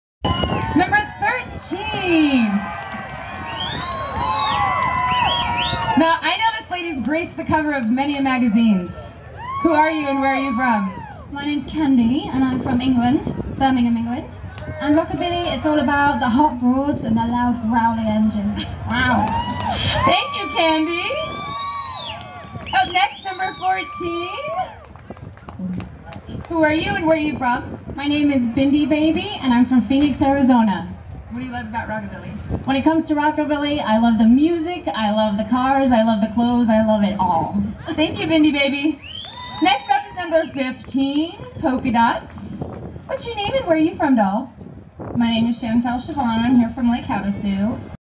Lake Havasu Rockabilly Reunion 2010
Kandy and Bindi Baby in the pin up contest